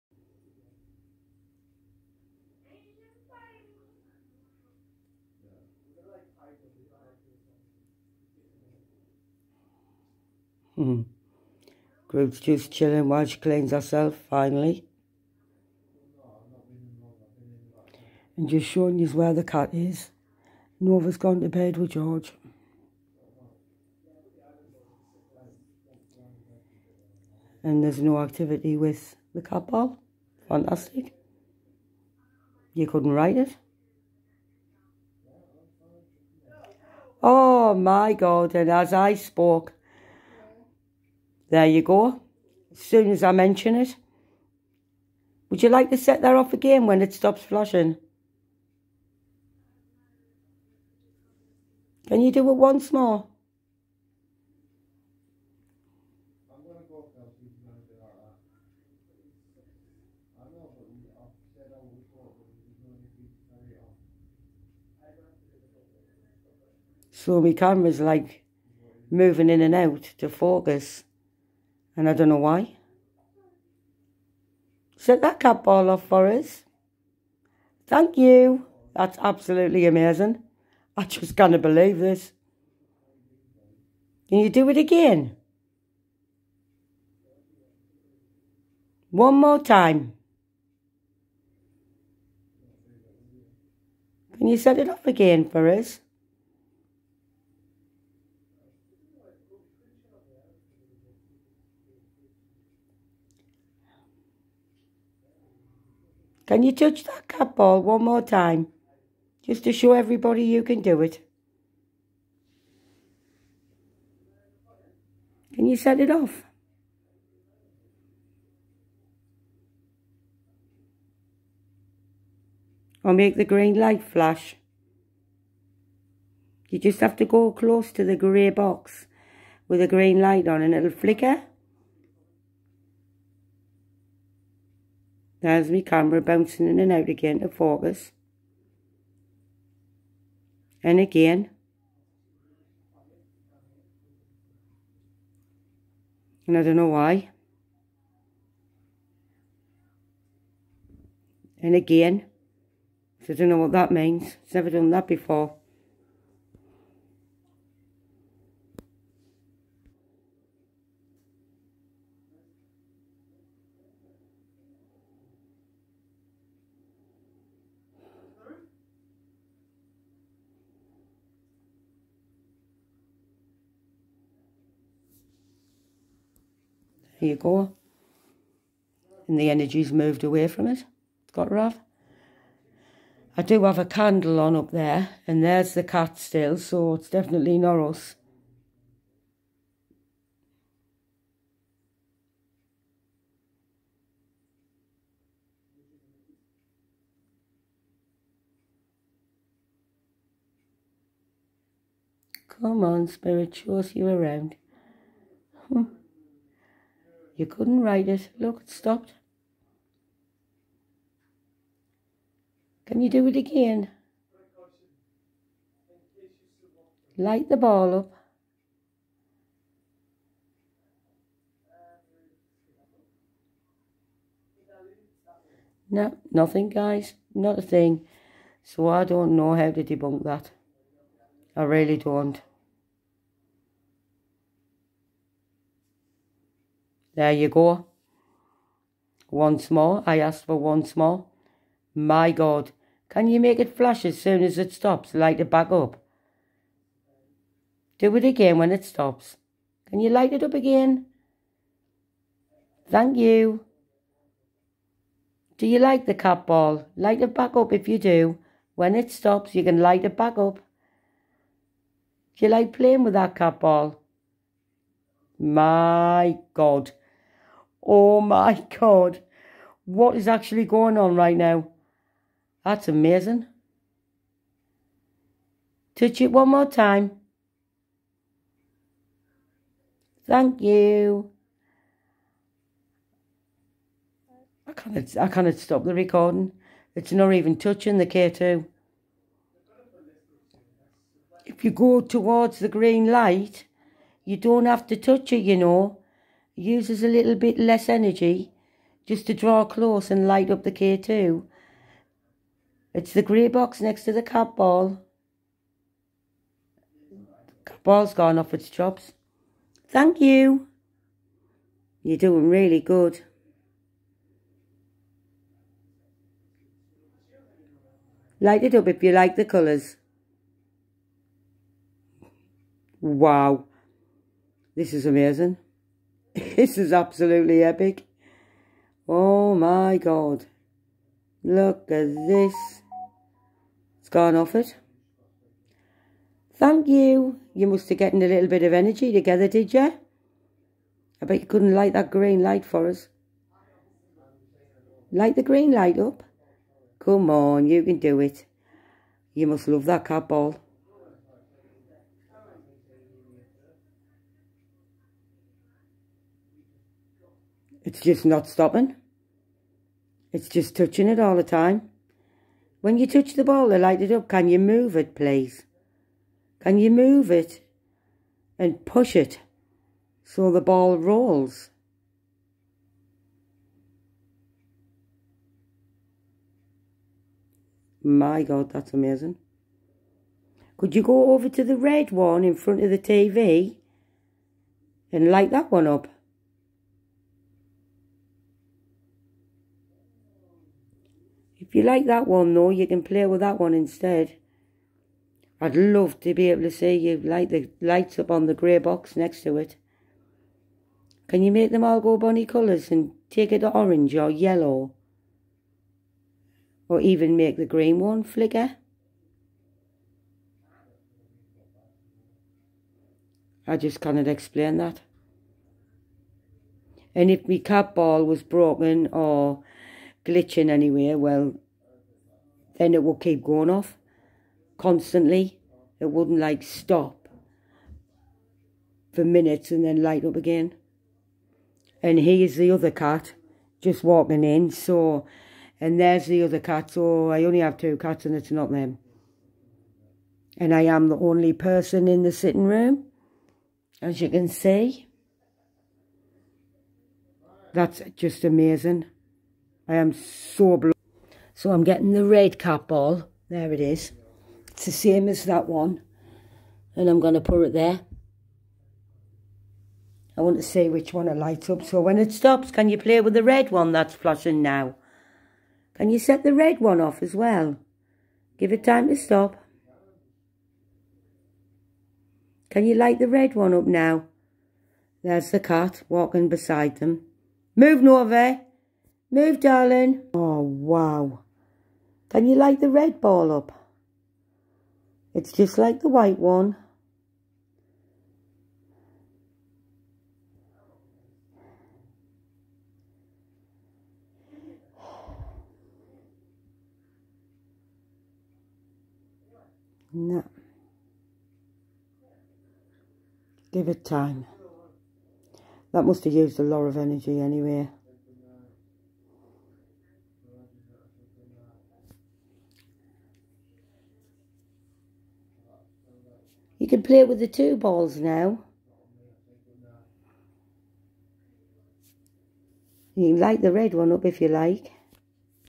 Eerie Encounters: PARANORMAL Communication in My Home with K2 and Cat Ball!(1)
so i set up my tools and i got spirit communication using my cat ball omg i can not believe this, i knew i could feel something